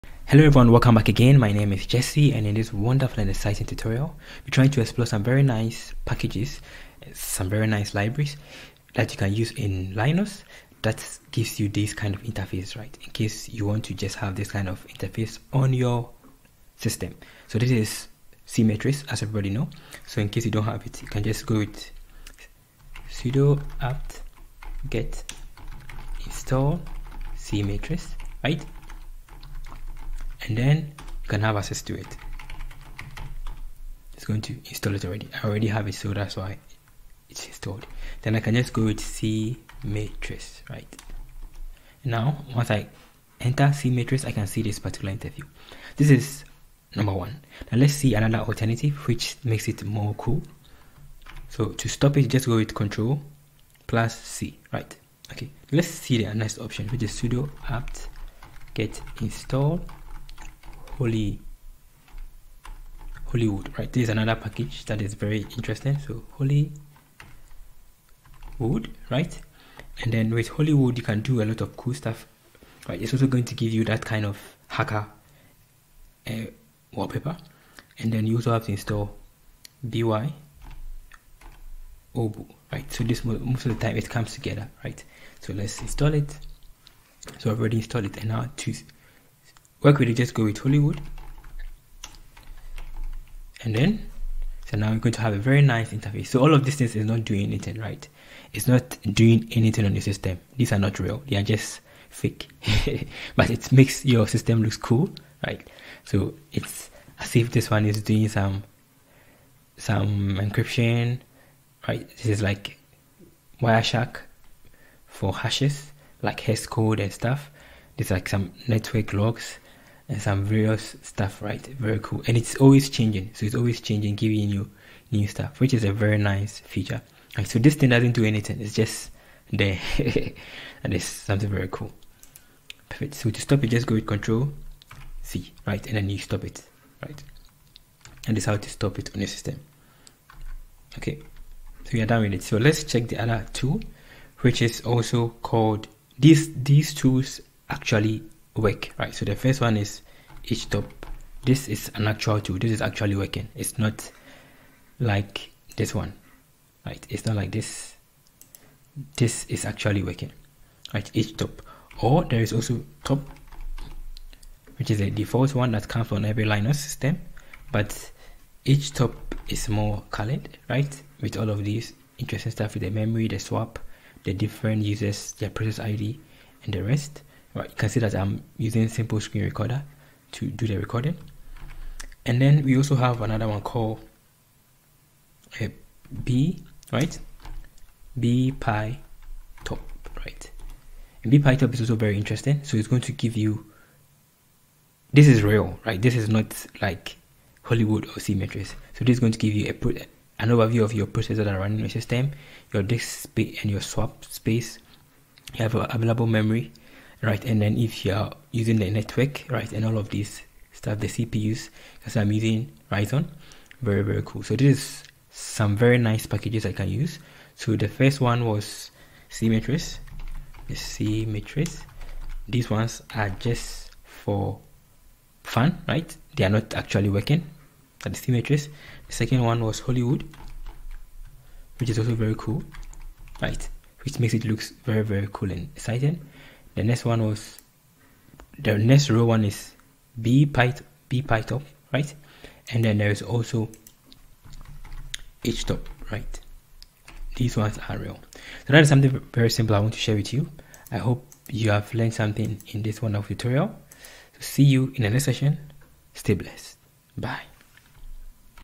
Three C-matrix Alternatives For Linux
In this tutorial we will explore 3 alternatives to cmatrix.

JCharisTech